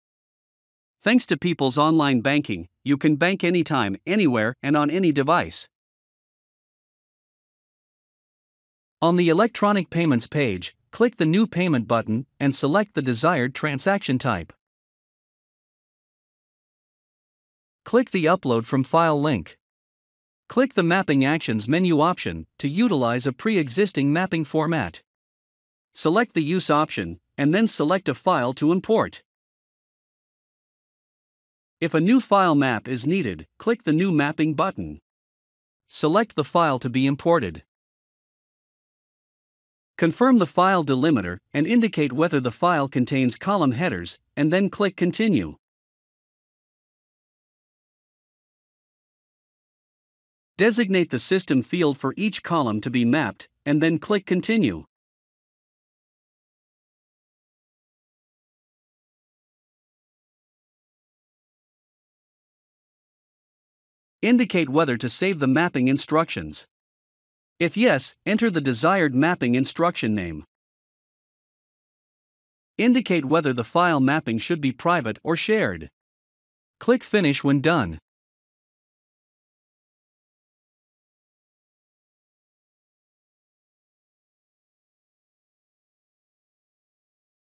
Business Online and Mobile Banking - How to Import an ACH File and ACH File Maps
How to import and map your ACH file imports. This demo video is recorded from a desktop view. Mobile device view may look slightly different.